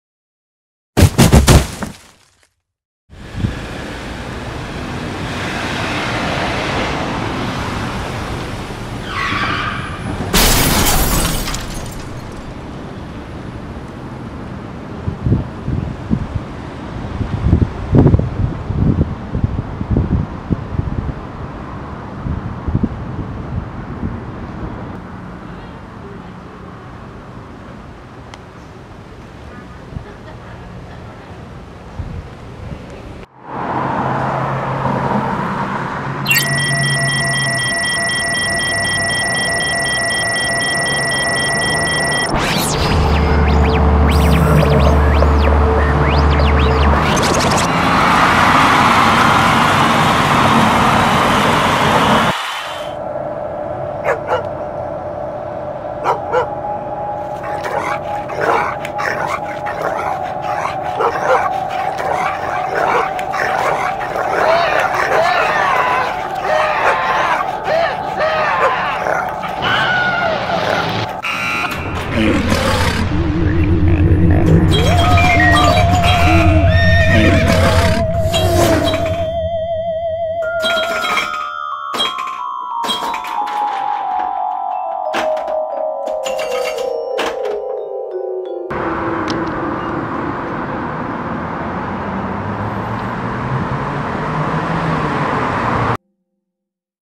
Bump It Up | Elliott Lytle
Made for Seattle Minute Movies | May 2021 | Theme: Bump
Credit: Elliott Lytle

For more information about Minute Movies and how to submit your own